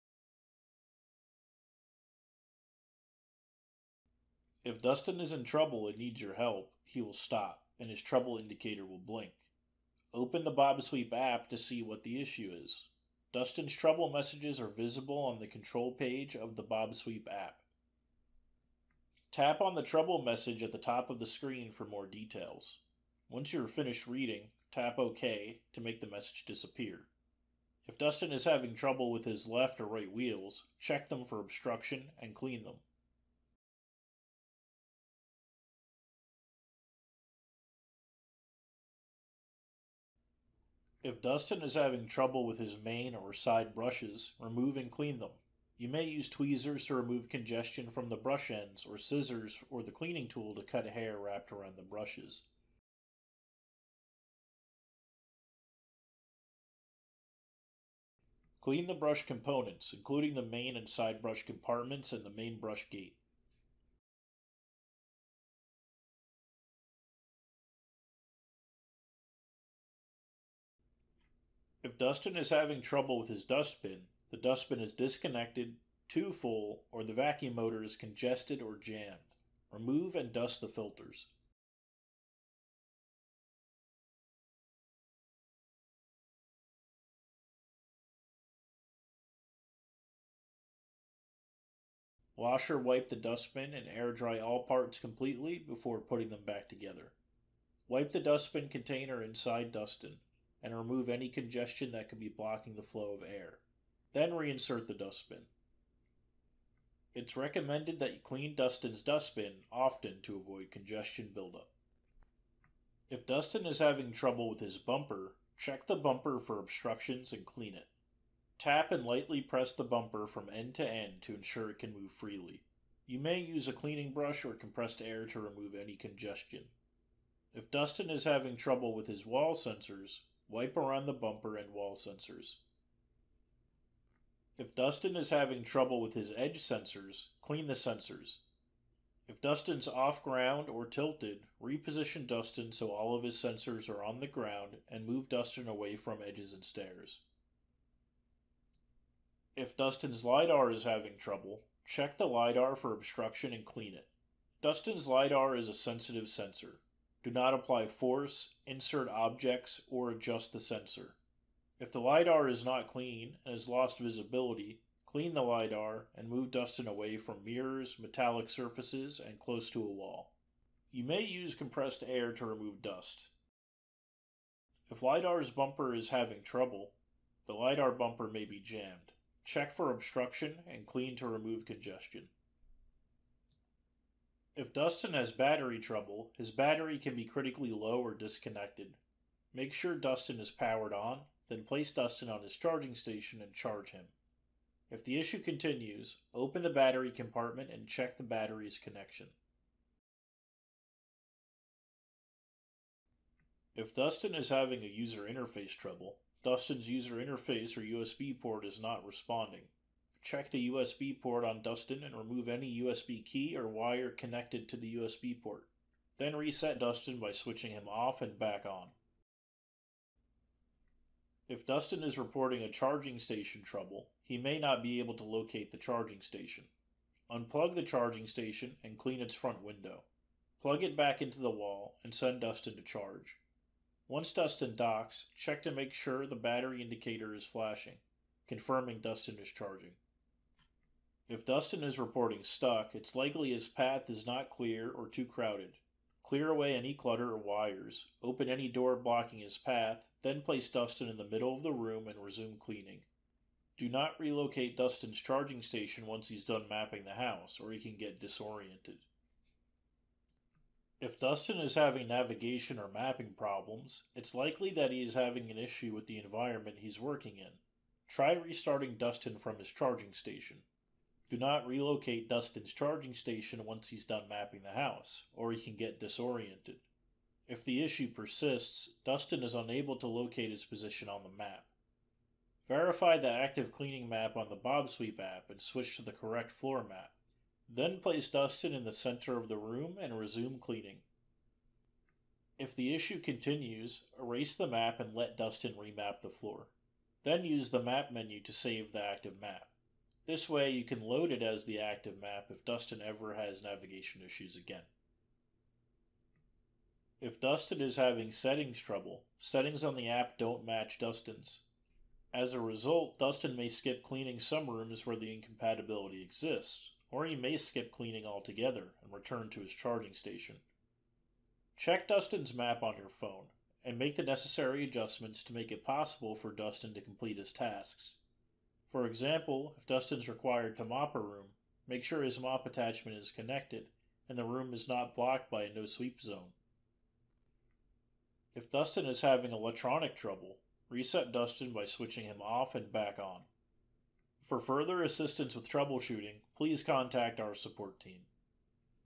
Troubleshooting | Dustin™
If Dustin is in trouble and needs your help, he will stop, and his trouble indicator will blink.

Open the bObsweep app to see what the issue is. Dustin’s trouble messages are visible on the Control page of the bObsweep app.

Tap on the trouble message at the top of the screen for more details.